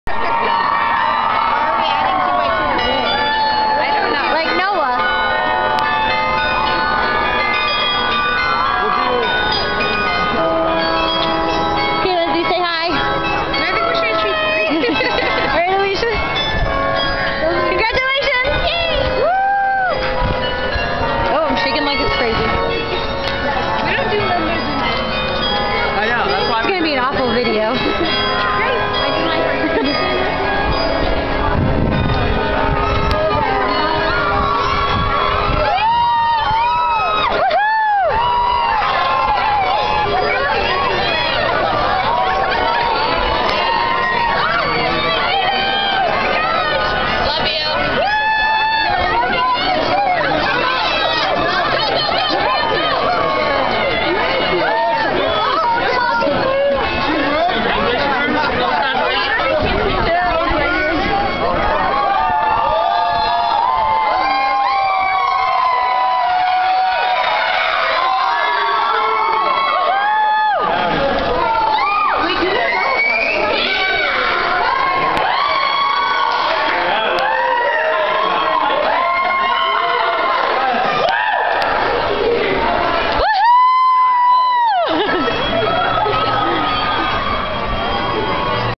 Walking through Campanile.MOV
After 4 and a half years of school, hundreds of dollars, sleepless nights, a semester abroad, this short walk turns out to have been quite a journey.